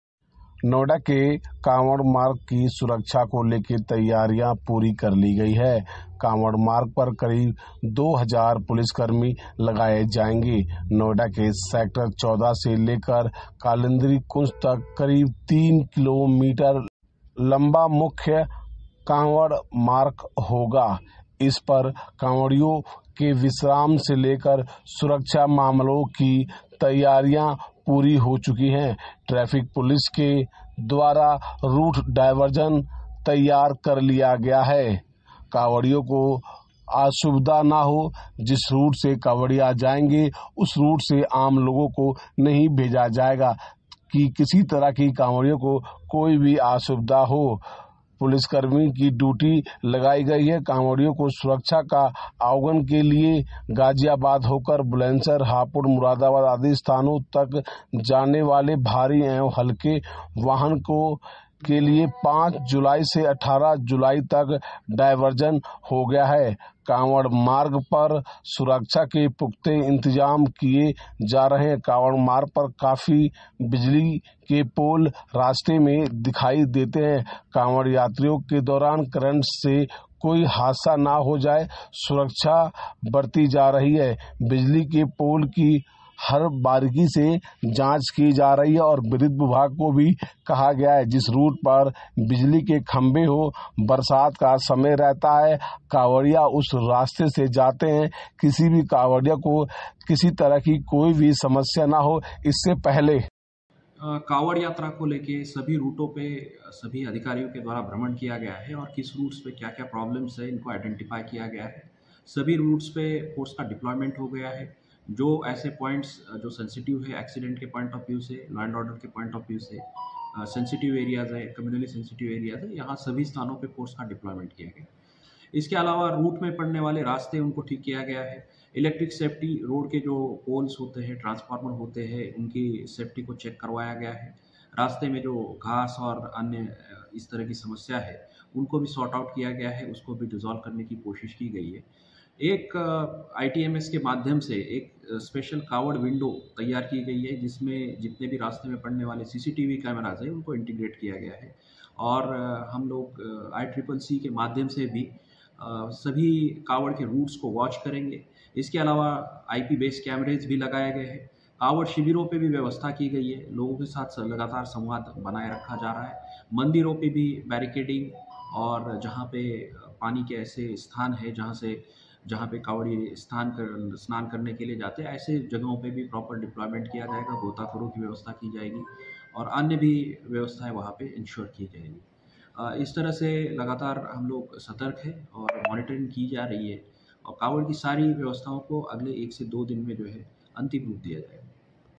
2000 पुलिस कर्मी….1000 कैमरों की निगरानी में होगी कांवड़ यात्रा की सुरक्षा, जानिए पुलिस का पूरा प्लान
&nbsp;

&nbsp;

विजय कुमार/नोएडा. नोएडा में कावड़ मार्ग की सुरक्षा को लेकर तैयारियां पूरी कर ली गई है. कावड़ मार्ग पर करीब 2000 पुलिसकर्मी लगाए जाएंगे. नोएडा के सेक्टर 14 से लेकर कालंदी कुंज तक करीब 3 किलोमीटर लंबा मुख्य कावड़ मार्क होगा. इस पर कांवरियों के विश्राम से लेकर सुरक्षा मामलों की तैयारियां पूरी हो चुकी है. ट्रैफिक पुलिस के द्वारा रूट डायवर्जन तैयार कर लिया गया है. असुविधा से बचने के लिए वैकल्पिक मार्गों का प्रयोग करने की सलाह दी गई है.

डायवर्जन वाले स्थानों पर ट्रैफिक पुलिसकर्मियों की ड्यूटी लगाई गई है. कावड़ियों के सुरक्षित आवागमन के लिए गाजियाबाद होकर बुलंदशहर, हापुड़, मुरादाबाद आदि स्थानों तक जाने वाले भारी एवं हल्के वाहनों के लिए 5 जुलाई से 18 जुलाई तक डायवर्जन रहेगा. कांवड़ मार्ग पर सुरक्षा के पुख्ता इंतजाम किए जा रहे हैं. कावड़ मार्ग पर काफी बिजली के पोल लगे हुए हैं इन पोल से कावड़ यात्रा के दौरान करंट से कोई हादसा ना हो जाए इसलिए सुरक्षा बरती जा रही है. बिजली के पोल पर लाल पट्टी के रिफ्लेक्टर टेप लगाने के साथ पन्नी भी लगाई गई है.

जारी किए गए है टोल फ्री नंबर

गौतमबुद्ध नगर के ज्वाइंट सीपी आनंद कुलकर्णी ने बताया ट्रैफिक पुलिस ने टोल फ्री नंबर जारी किए हैं. सेक्टर 14 स्थित चिल्ला पाउडर के पास स्थाई ट्रैफिक बूथ बनाया गया है. यहां से ट्रैफिक पुलिस कर्मियों की ड्यूटी की निगरानी रखी जाएगी. कावड़ का जत्था गुजरने के दौरान ट्रैफिक पुलिस कर्मी ट्रैफिक को रोककर कांवरियों को रास्ता दिखाने का कार्य करेंगे. ज्वाइंट सीपी आनंद कुलकर्णी ने बताया सभी पुलिसकर्मियों की ड्यूटी तय कर दी गई है. पुलिसकर्मी अपनी ड्यूटी समय-समय पर जहां-जहां लगाई गई है वहां पर मुस्तैद होकर रहेंगे समय-समय पर अधिकारी द्वारा जांच की जाएगी।